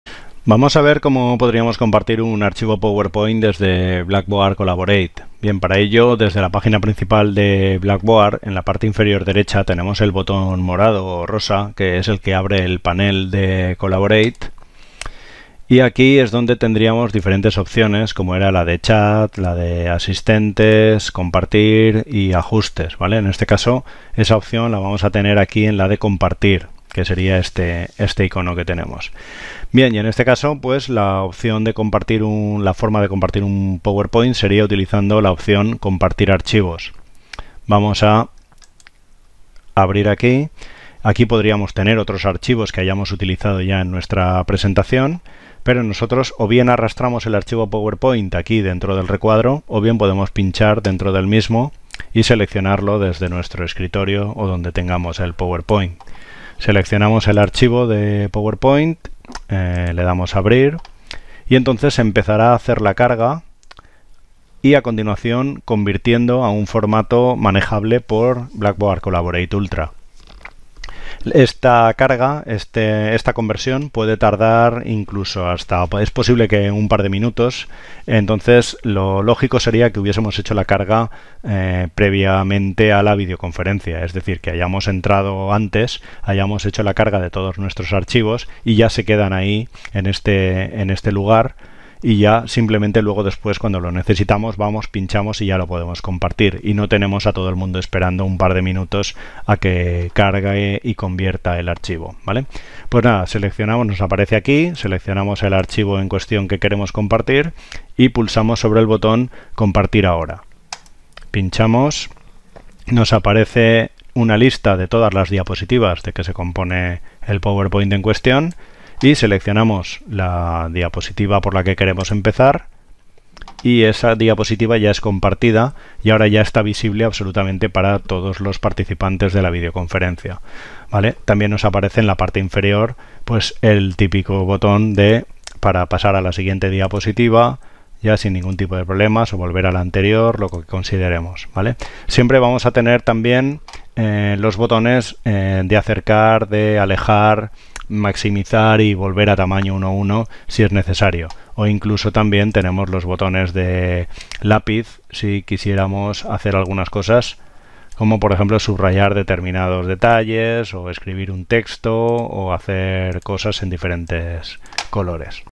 Compartir un Powerpoint en Blackboard Collaborate Ultra en la UV
Compartir un Powerpoint en Blackboard Collaborate Ultra. Vicente Bresó. Universitat de València.